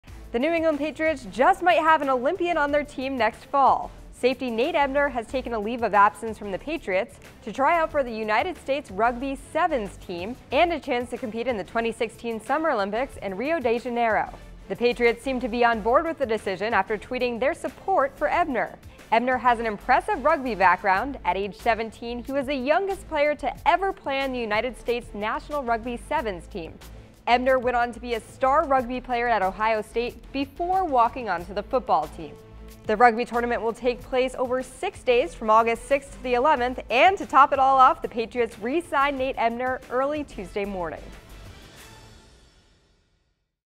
Nate Ebner To Try Out For USA Rugby Olympic Squad
New England Patriots Safety Nate Ebner is taking leave from the team to try out for Team USA Rugby in hopes of joing the team for the 2016 Summer Olympics in Rio.